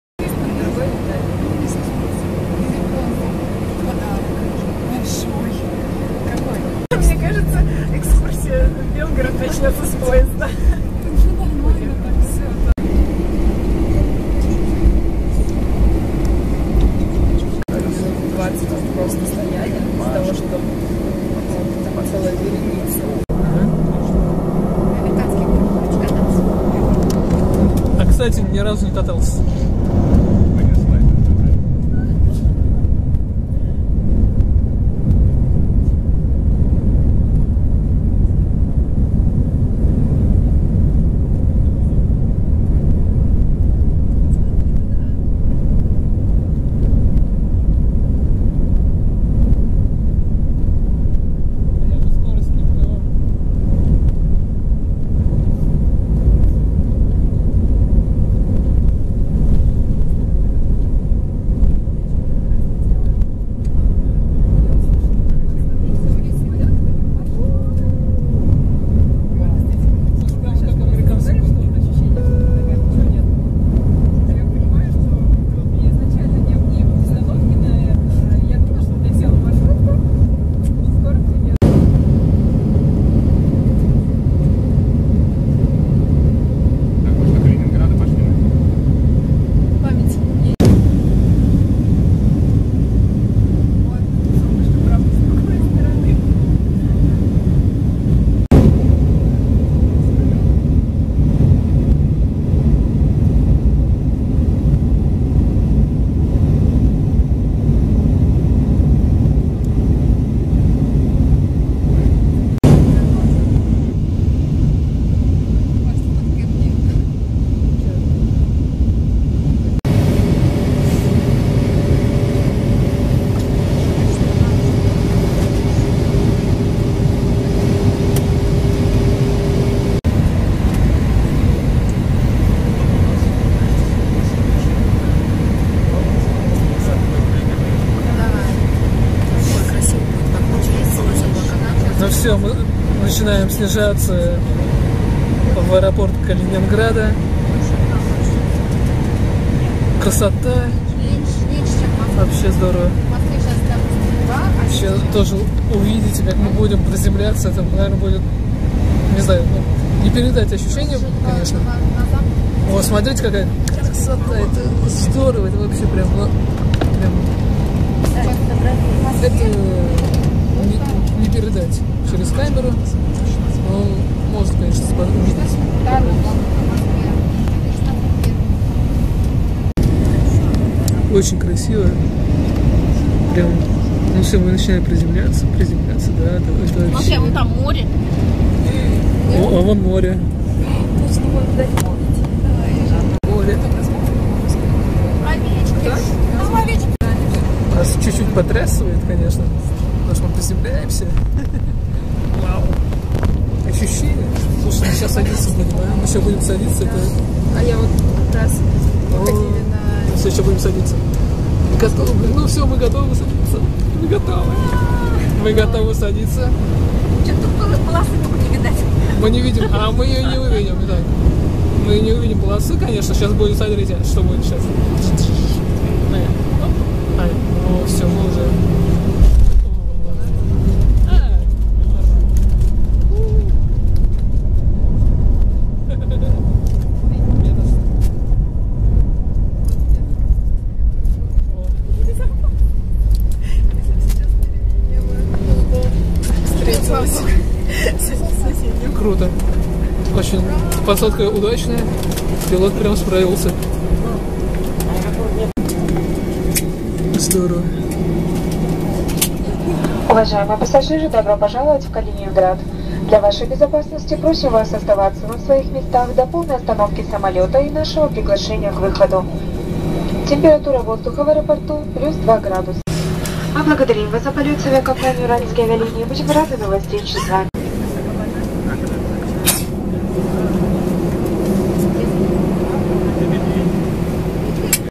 Мой первый полёт на самолёте (21.12.16)
За 26 лет ни разу не пользовался услугами авиалиний. Этот день настал 21 декабря 2016 года.

Для мобильных репортёров провели экскурсию по городу Калининград и мастер-класс от известных журналистов:

Интернет-проект "Мобильный репортёр". Телеканал "Россия 24"